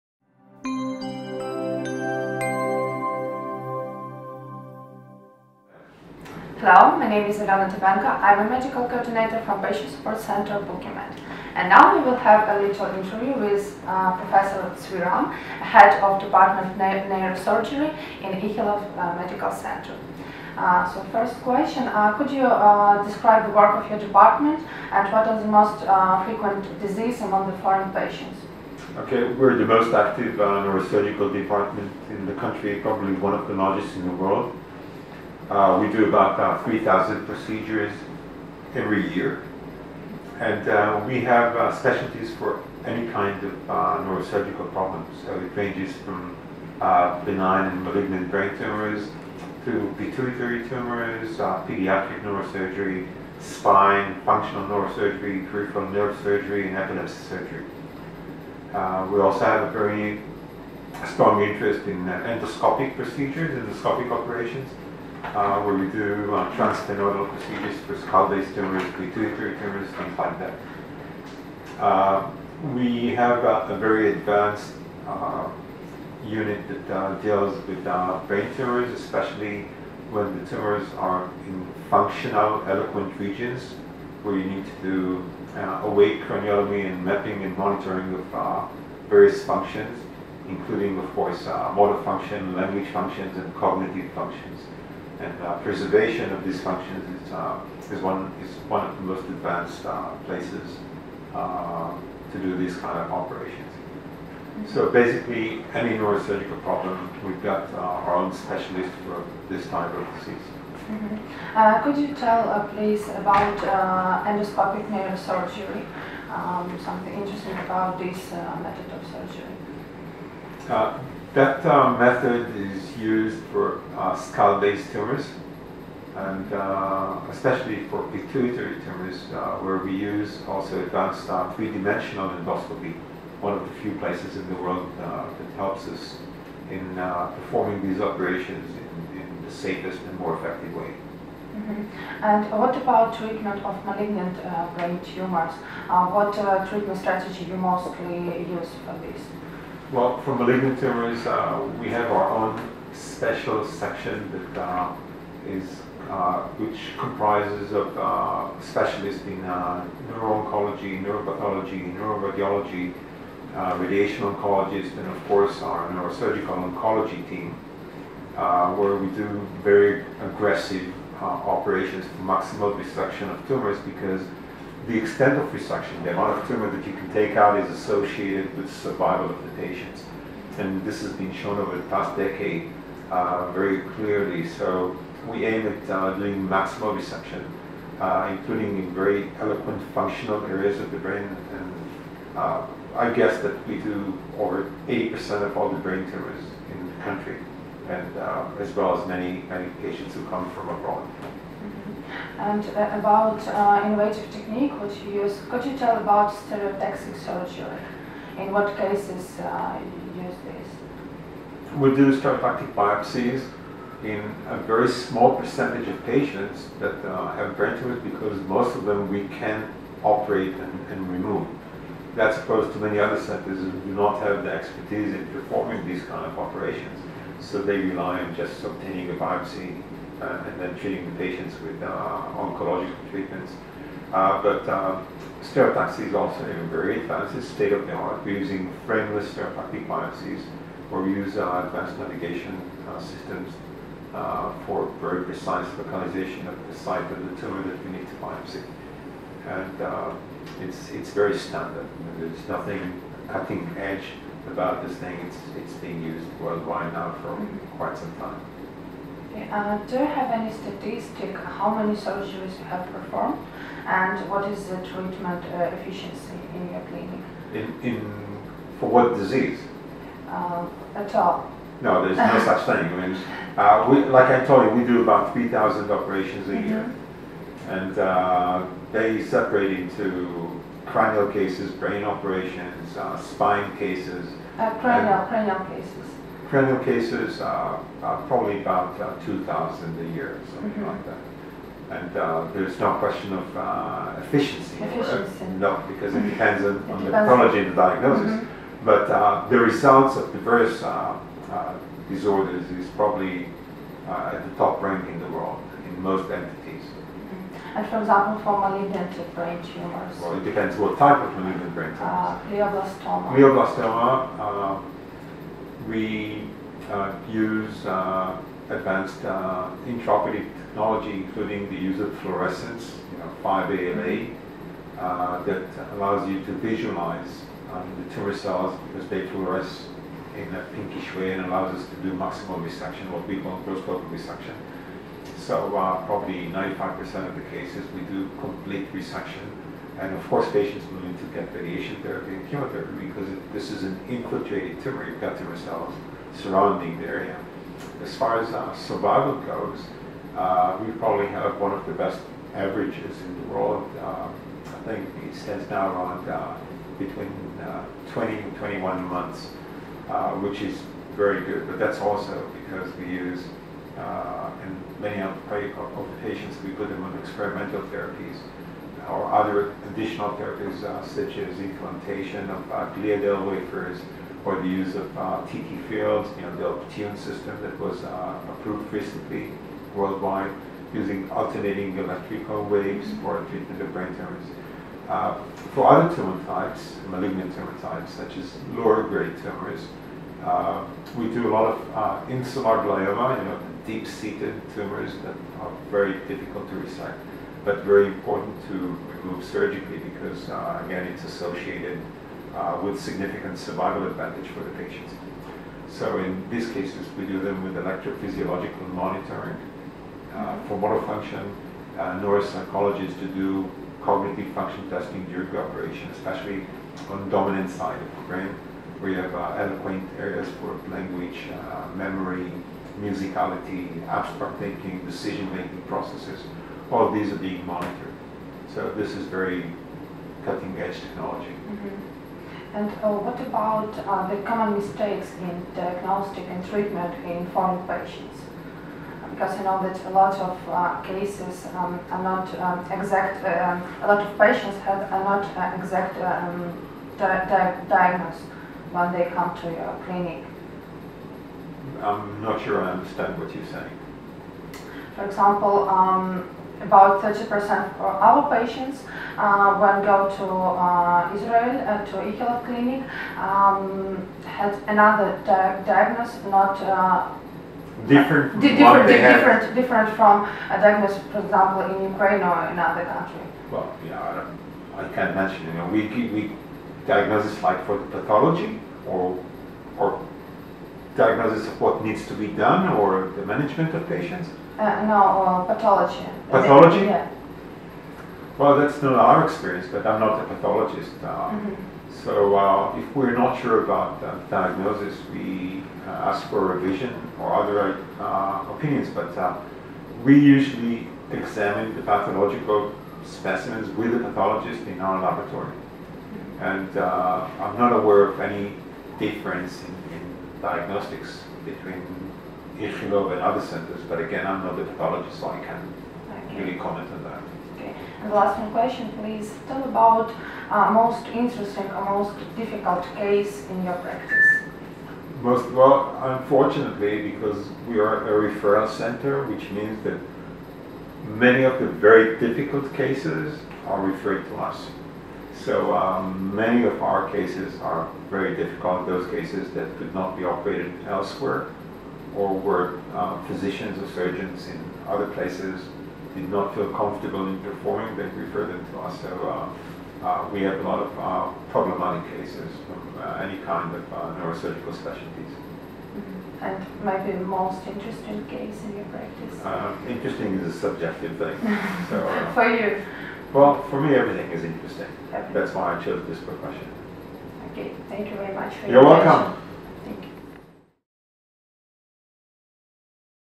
The unique interview with one of the top neurosurgeons of Israel, Professor Zvi Ram
Head of the department of neurosurgery clinic Ichilov Professor Zvi Ram for international patients Support Center Bookimed about:
- Endoscopic neurosurgery;
- Treatment of malignant brain cancer;
- Stereotactic surgery;
- Common diagnostic and treatment errors  (about foreign patients).